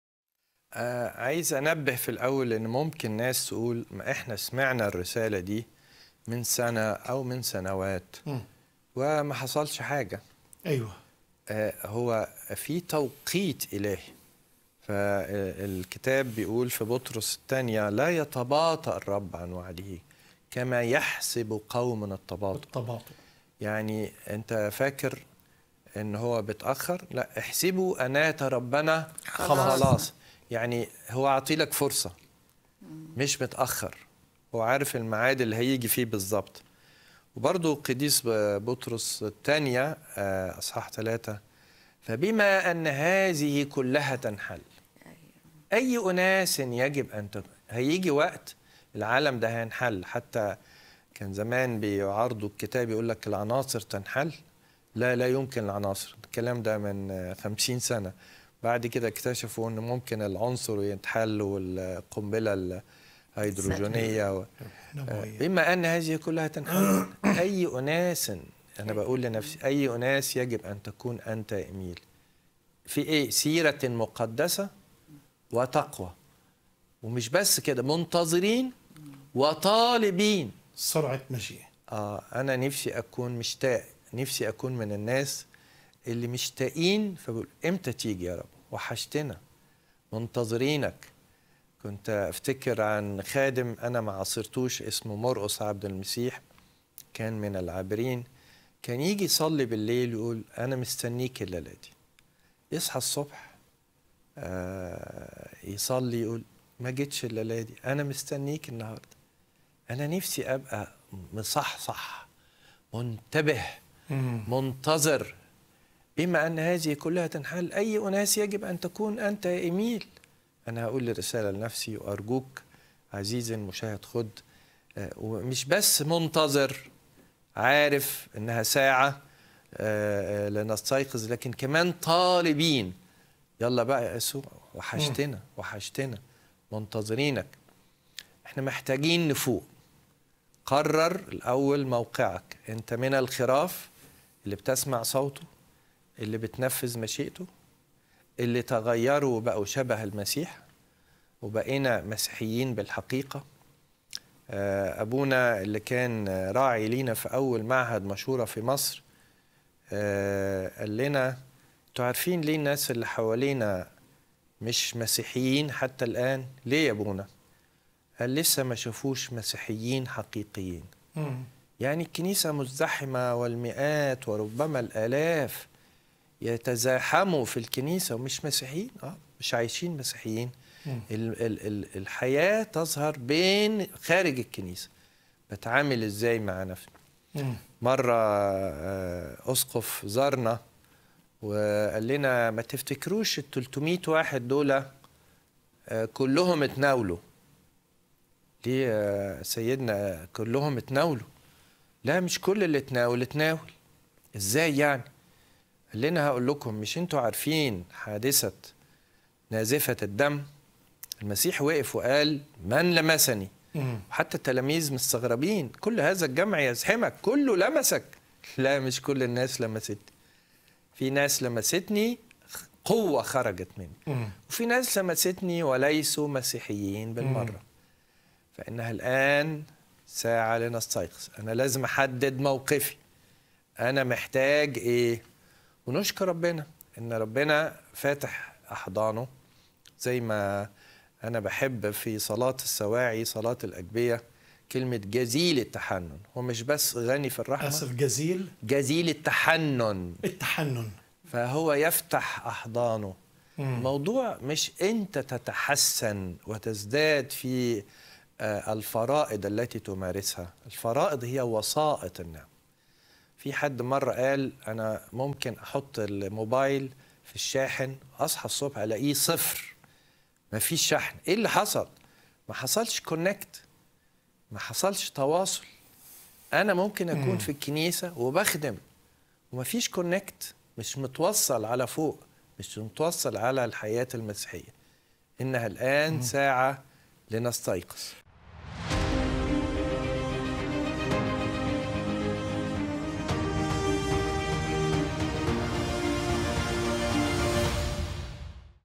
لا يتباطأ الرب عن وعده
ممكن يكون لسان حالك، ان فكرة مجيء الرب بعيده، وانه من زمن طويل بيقولوا انه جاي ولسه لحد دلوقتي.
الكتاب المقدس بيقول، انه الرب لا يتباطأ عن وعده
وانه مجيئه حقيقة مؤكده، ومفاجأة
لكن الأمر الآخر، هو انه حياتنا نحن مثل بخار، فقط تنتهي فى أي لحظة دون سابق انذار

فكن مستعد

🔗لمتابعة حلقة "إنها ساعة لنستيقظ" كاملة، الرابط فى أول تعليق